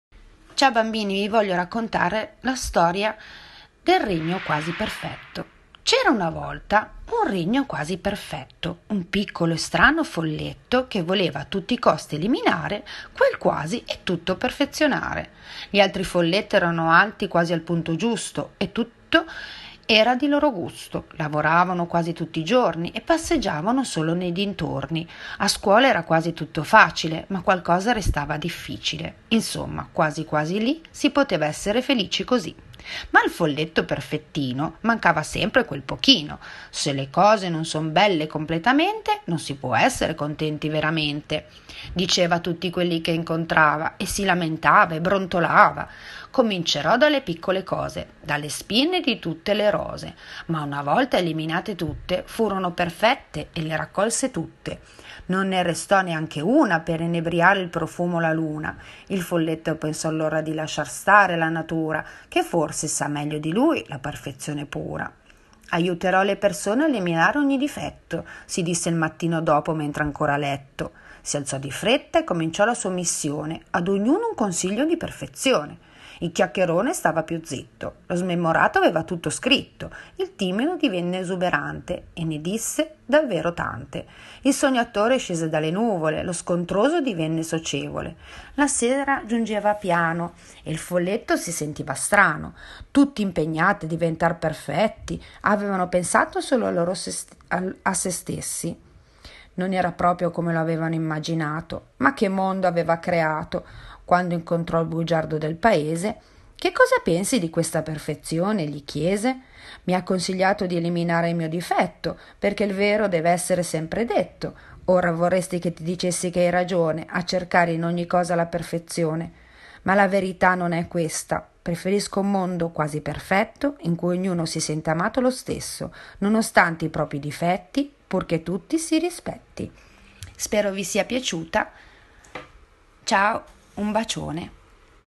Il regno quasi perfetto - Maestra Alessandra Burelli
Care bambine e cari bambini, mamme, papà e nonni,

è così è arrivato questo virus che porta la corona, ma di certo non è un re, e nemmeno una persona. È un tipaccio piccolino, come narra il buon Piumini. Non bisogna aver paura, stando a casa stretti stretti, questo virus non farà dispetti.

Questo spazio dedicato, che puoi guardare con chi vuoi, è pensato per tutti voi.

Le maestre han pensato di dedicarvi delle letture, perché le parole sono cure e allontanano le paure. Con prudenza e attenzione, vinceremo contro questo gran birbone. Quando tutto sarà terminato e la prova avremo superato, tutti insieme torneremo ad abbracciarci tutto d’un fiato.